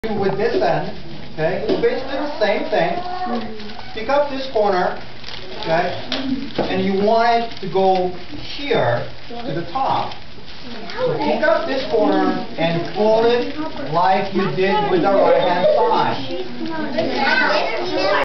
How to Fold Kabuto (Samurai Helmet) Origami 07/13
I was kindly invited by Ms. Carol Atkinson from North Beach Elementary School to join her class to celebrate the Japanese Children's Day (May 5). This short clip is part of the 13-part series in which I tried to teach them how to fold kabuto (samurai helmet) origami.